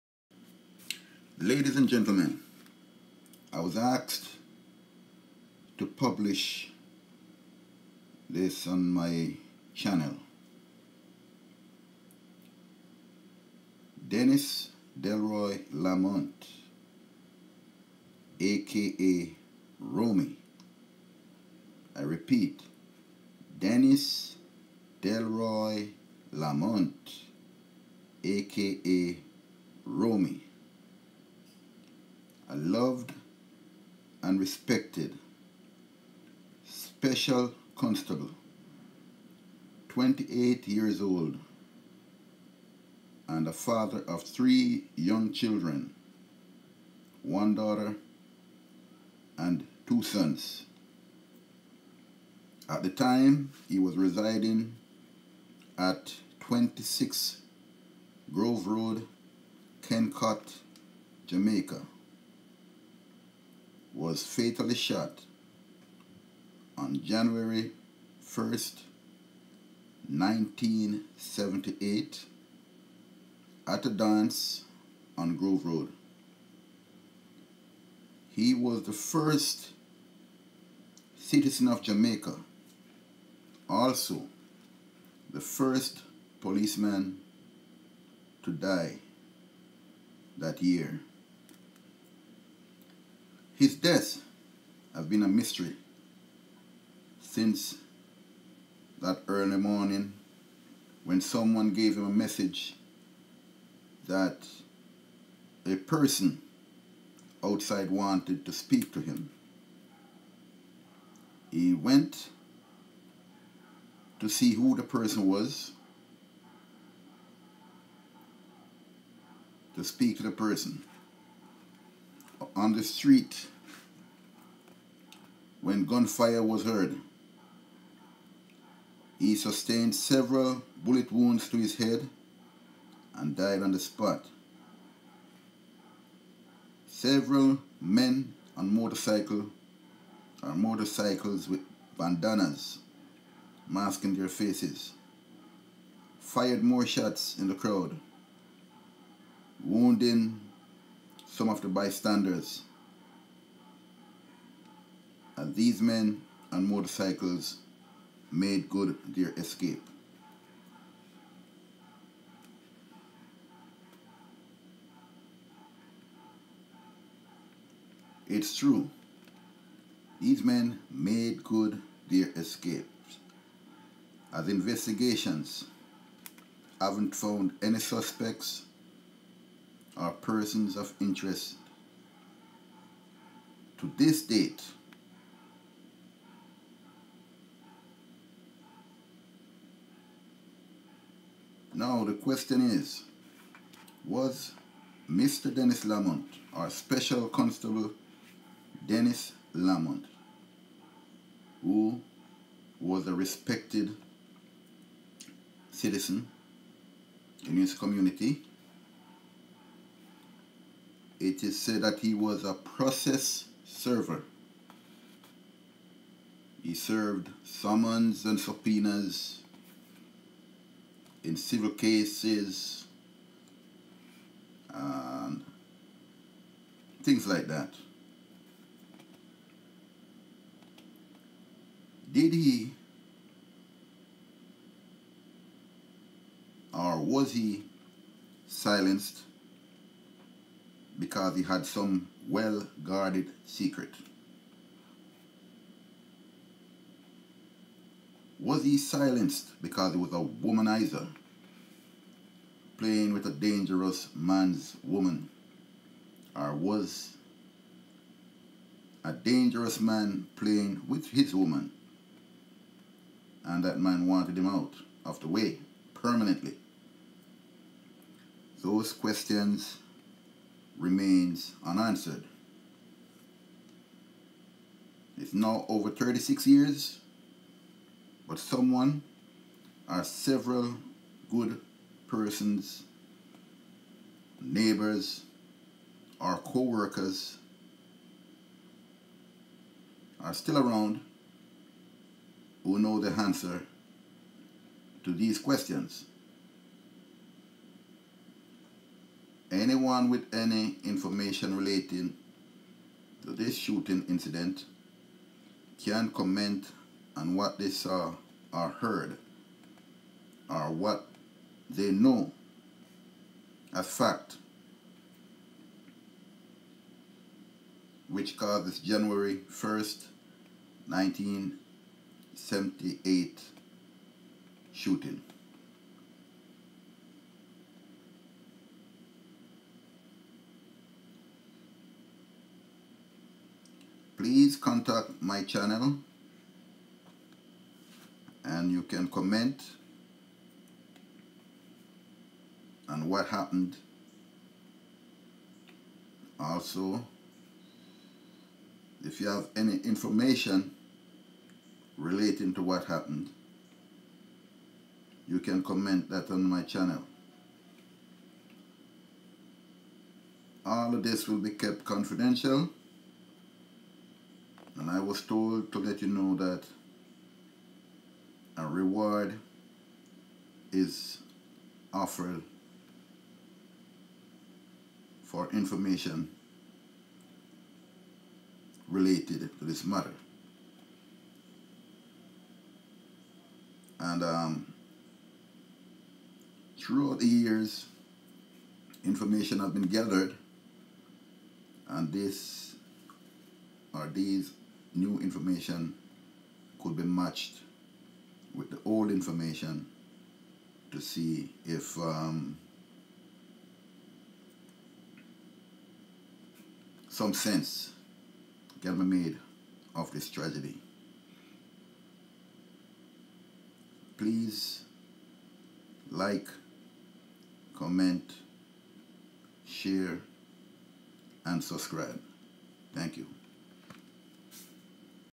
$10,000.00 (US)REWARD OFFERED FOR INFO.
The silencing of a model citizen and father on New Years Day 1978